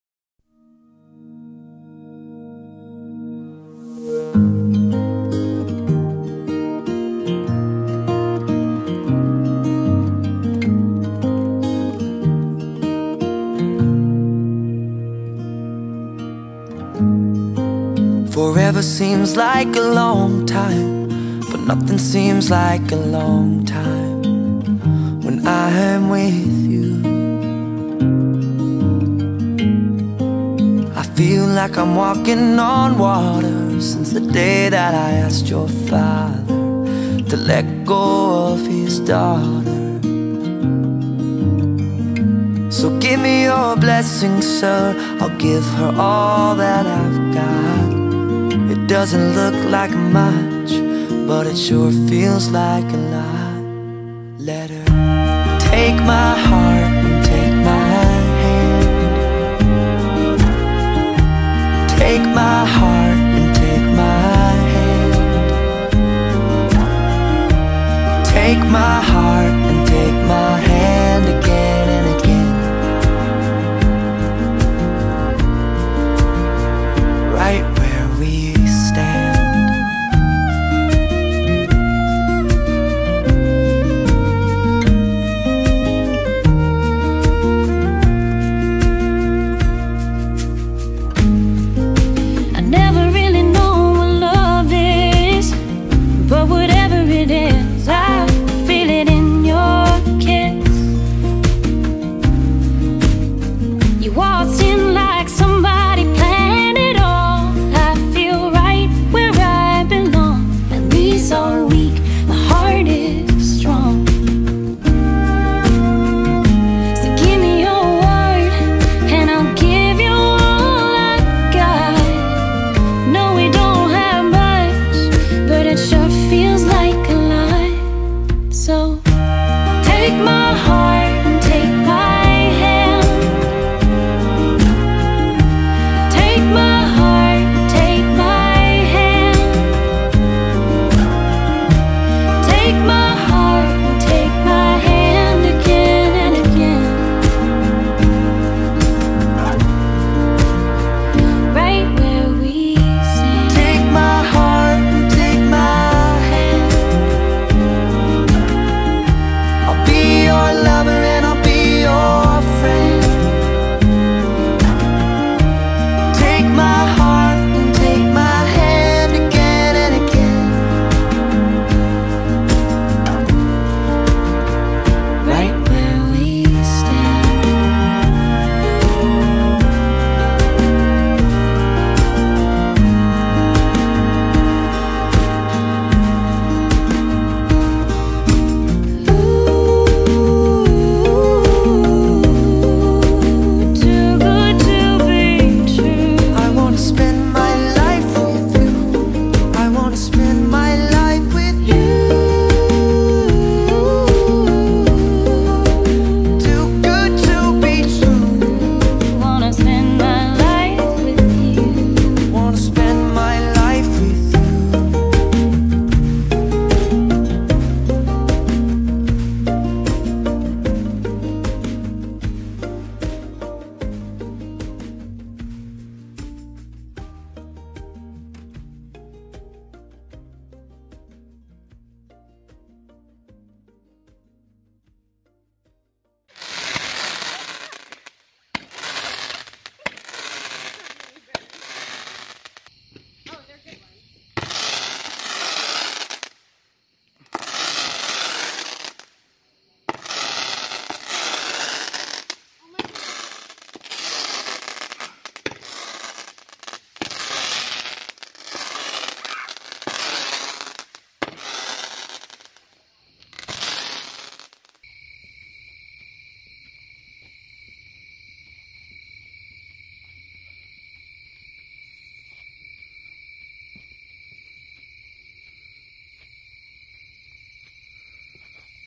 Brandon & Morgan Engagement 9/6/2020
Our engagement at Riverside on 9/6/2020!
Thank you to our friends and family for making the evening so special! A special thanks to Mike Needham for the drone footage and editing. We couldn't be happier!